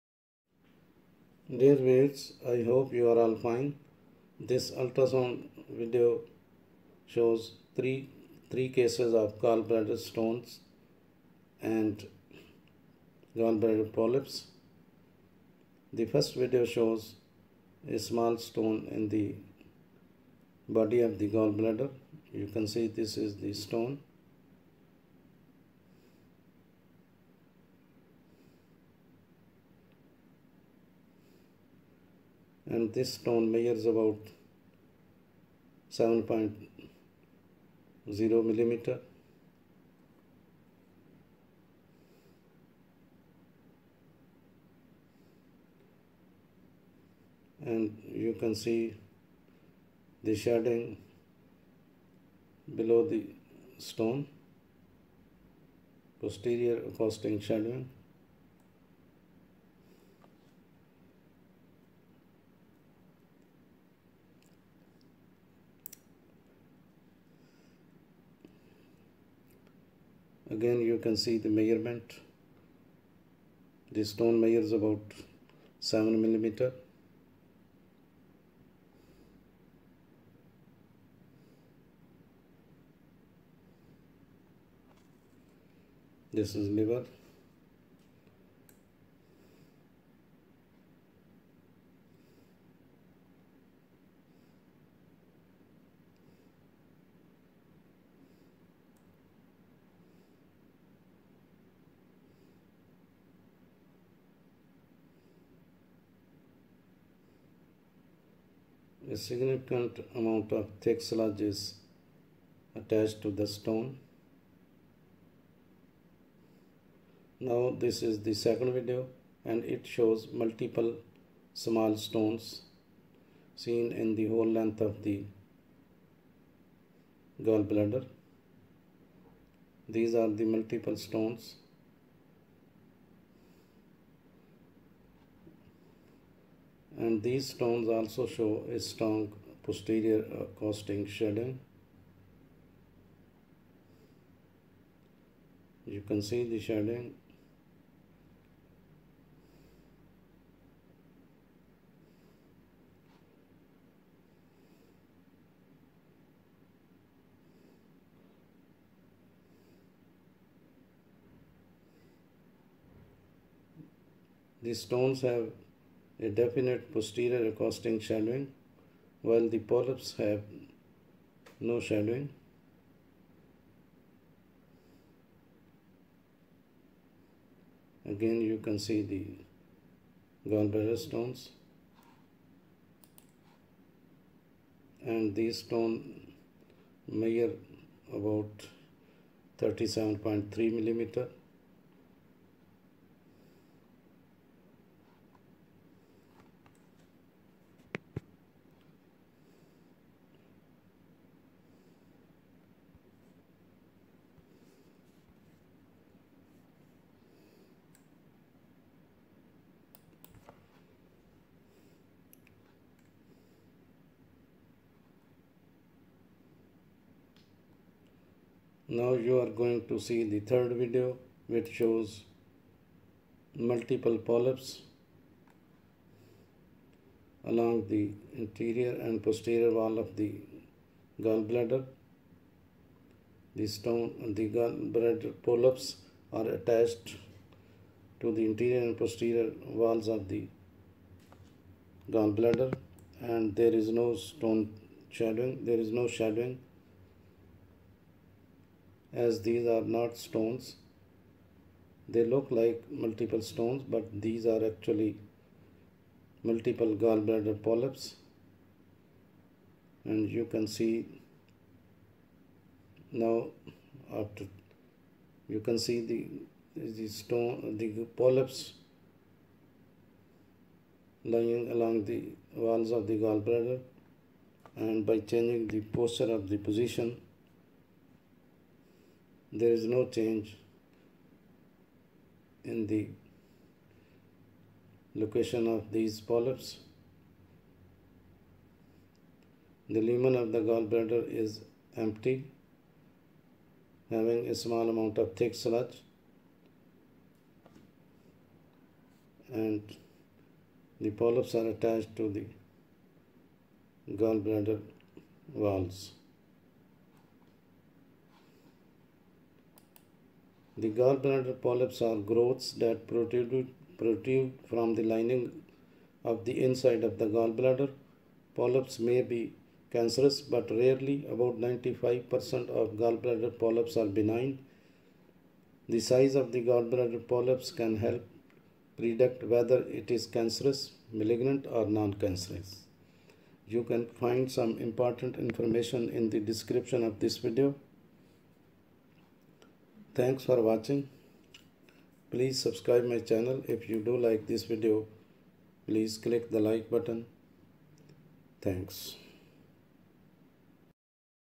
Ultrasound Video showing Gallbladder Stones and polyps.
This video shows Gallbladder Stones and polyps.
Gallbladder polyps are growths that protrude from the lining of the inside of the gallbladder. Polyps can be cancerous, but rarely. About 95% of gallbladder polyps are benign. The size of a gallbladder polyp can help predict whether it's cancerous (malignant) or noncancerous (benign).
Surgery is necessary if polyps cause symptoms or are larger than 1 cm. Surgery is recommended when a polyp has grown by 2 mm or more since the last checkup. The treatment involves surgical removal of the gallbladder.
Gallbladder polyps are elevated lesions on the mucosal surface of the gallbladder. The majority are benign, but malignant forms are also seen.
Gallbladder polyps are common, seen in up to 9% of the population.
Over 90% are polyps are benign, and the majority of these are cholesterol polyps.
Cholesterol polyps are most frequently identified in patients between 40-50 years of age and are more frequently seen in females.
                 Benign polyps are about 95% of all polyps
Cholesterol polyps: more than 50% of all polyps
Adenoma: 30%, possibly premalignant
Inflammatory polyps: 10%
other rare entities (see benign tumors and tumor-like lesions of the gallbladder)
Malignant polyps: 5% of all polyps
Adenocarcinoma: 90% of malignant polyps
other rare entities including metastases to gallbladder squamous cell carcinoma angiosarcoma
Polyp more than 10 mm have increased risk of malignancy, cholecystectomy recommended.